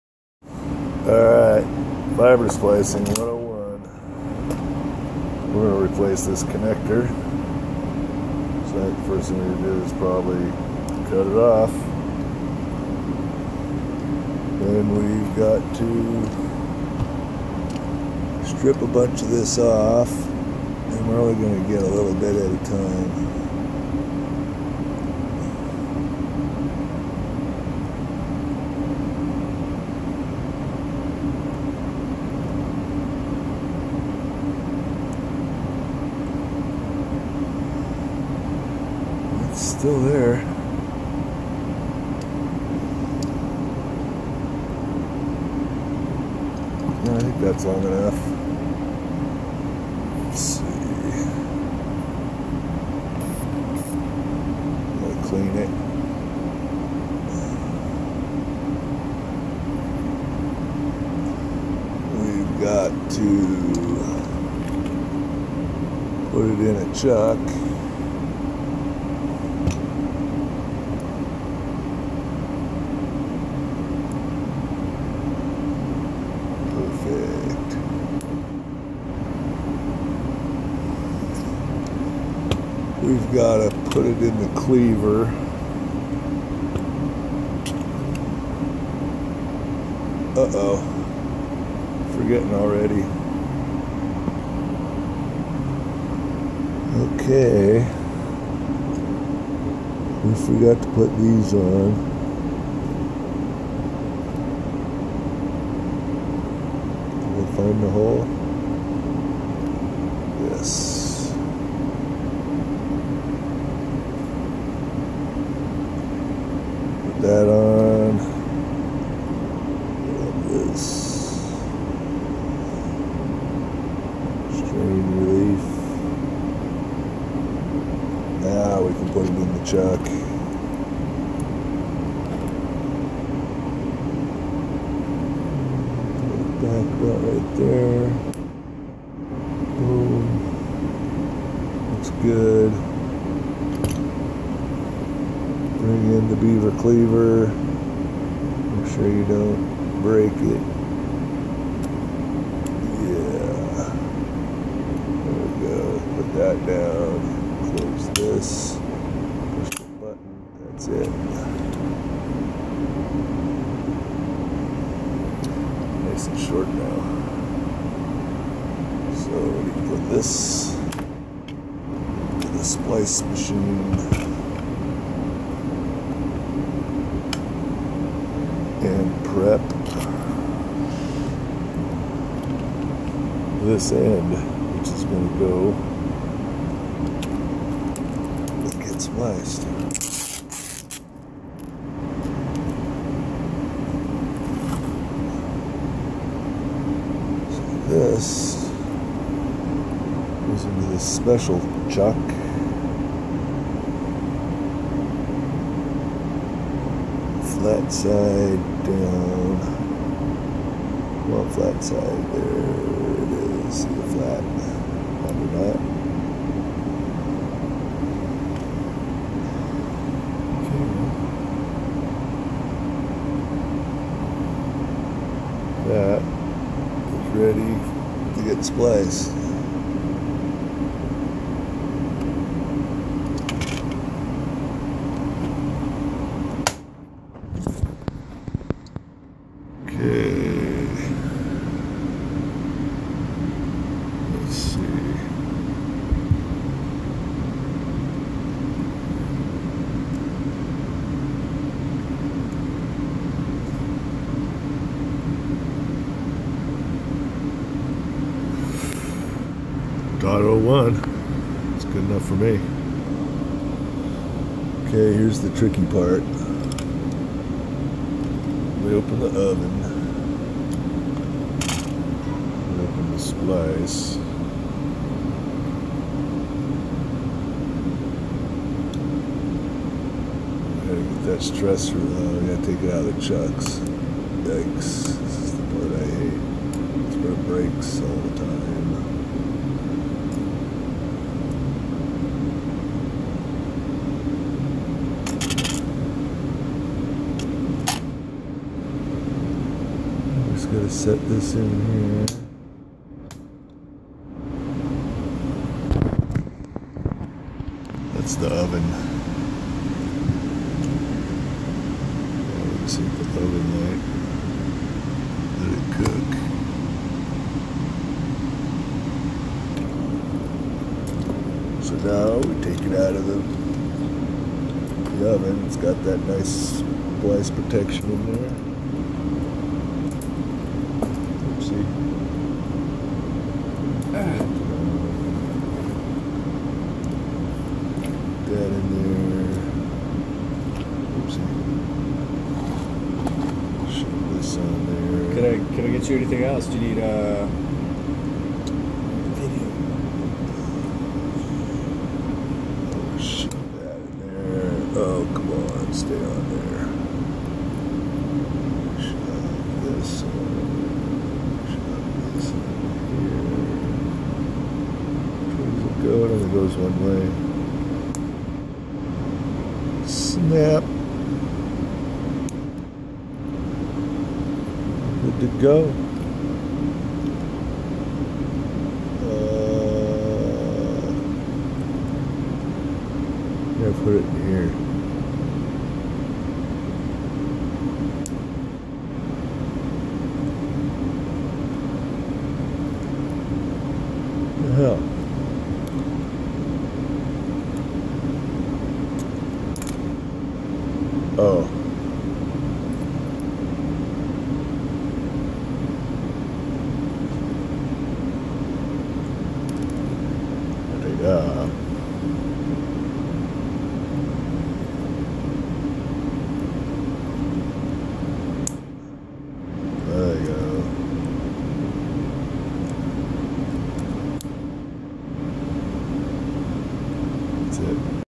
Fusion Splicing
Replacing old D4 connector with a new SC.